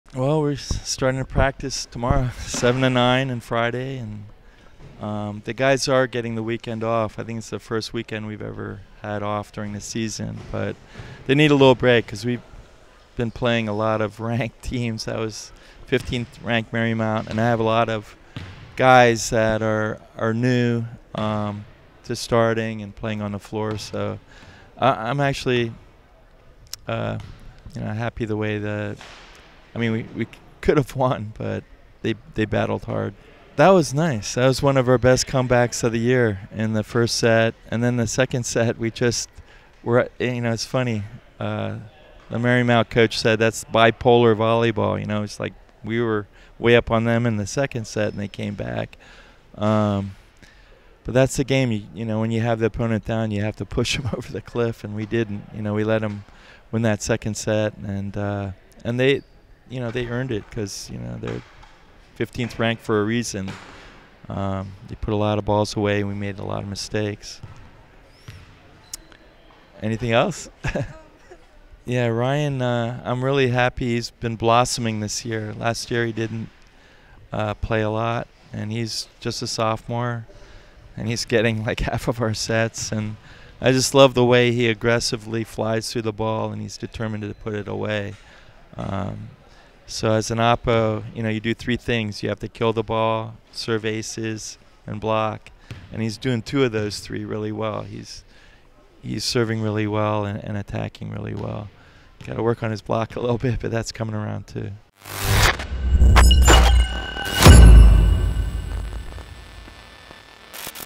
Men's Volleyball vs. Marymount | Postgame Interview with Head Coach Aldis Berzins (2/22/2023)
Following a close match to Marymount, here's head coach Aldis Berzins with the goals and information on improvement as the team breaks this weekend.